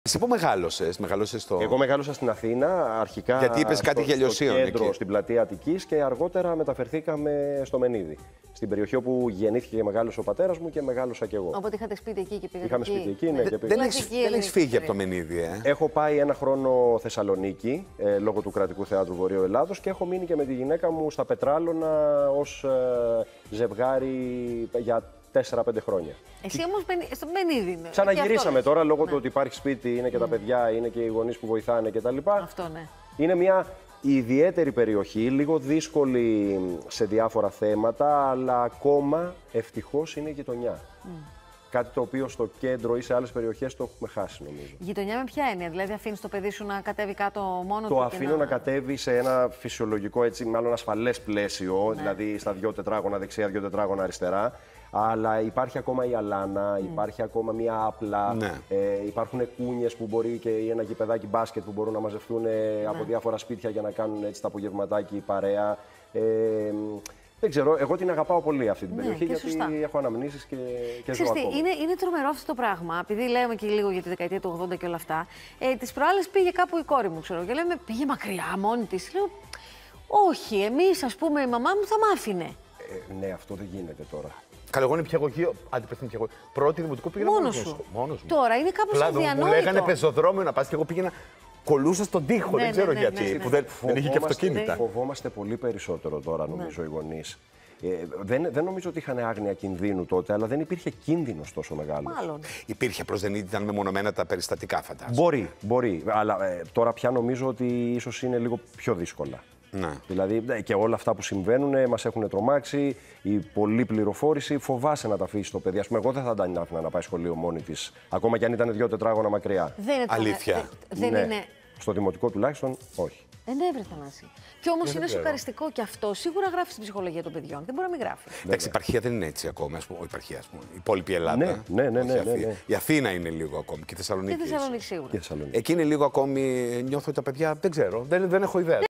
Μελέτης Ηλίας: «Ζω ακόμα στο Μενίδι γιατί είναι γειτονιά» | 06/09/2022 | ΕΡΤ
Ο δημοφιλής ηθοποιός μιλά για το πως έχουν αλλάξει τα πράγματα από την εποχή που ήταν ο ίδιος παιδί μέχρι σήμερα που σκέφτεται ως γονιός, στους δρόμους των πόλεων και στο αίσθημα ανασφάλειας που υπάρχει.